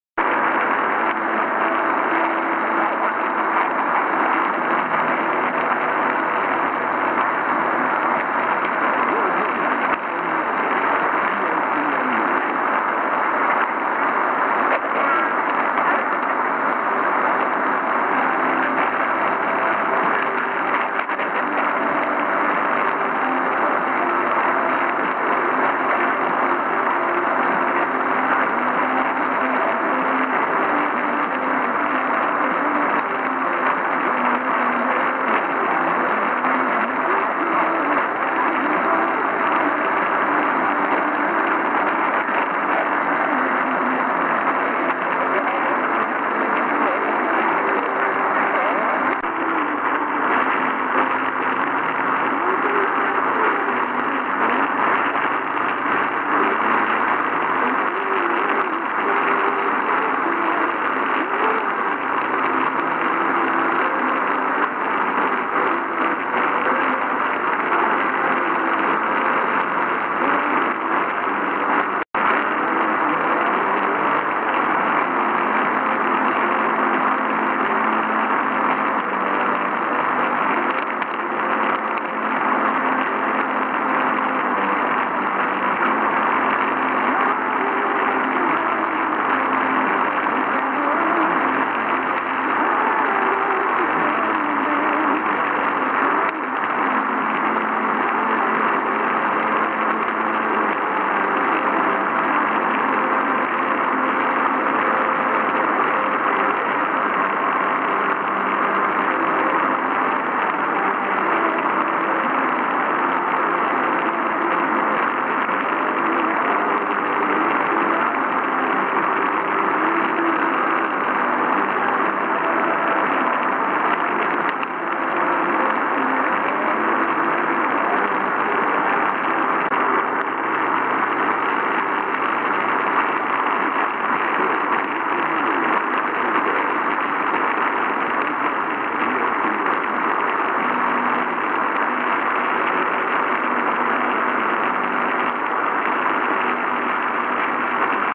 MW-DX: 590kHz - VOCM St. John's received in Bochum, Germany
from St. John's (4230km), received on 14 October 2014 at around 01:25 UTC / 03:25 CET, IDs "VOCM News" at 0:11 and "5-90 VOCM" at 2:18